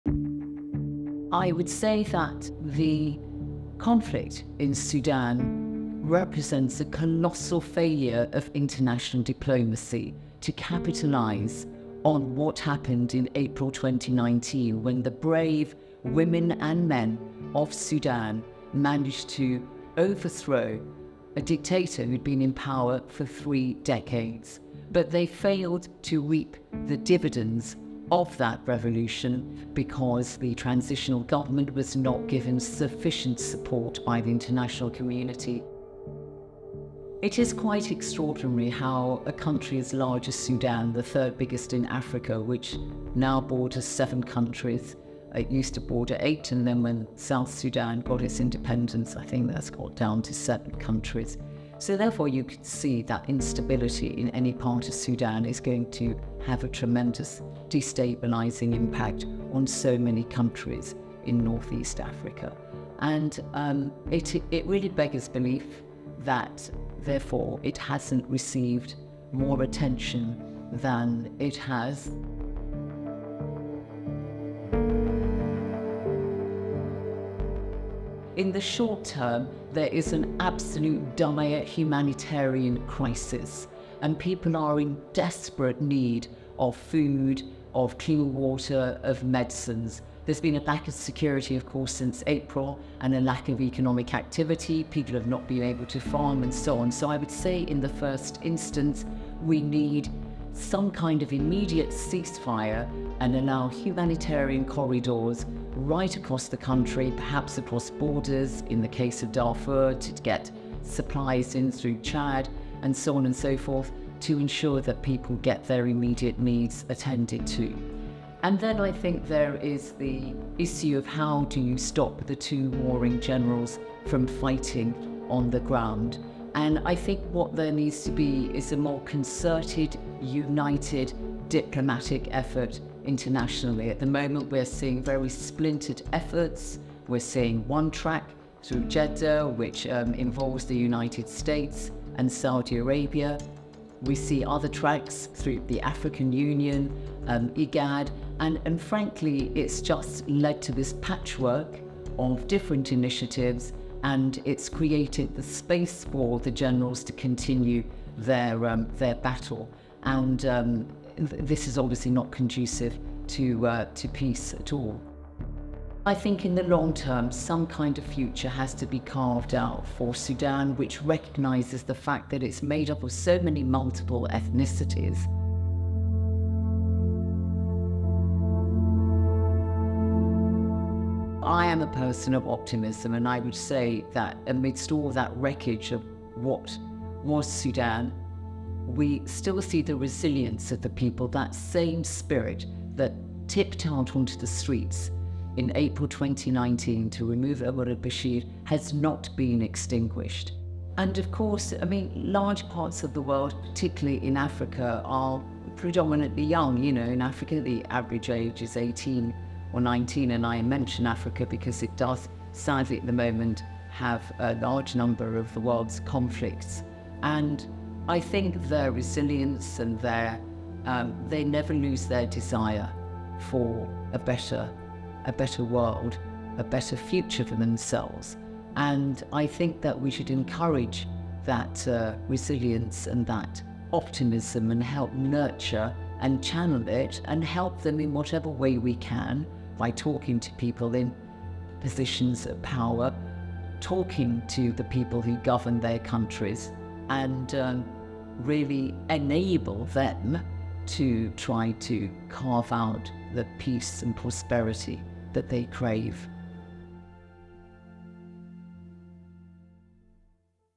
Zeinab Badawi: The War in Sudan
In this video, Crisis Group Trustee and President of the School of Oriental and African Studies (SOAS) at the University of London, Zeinab Badawi, speaks about the failure of international diplomacy to respond effectively to events in Sudan, especially following the break out of war in April 2023.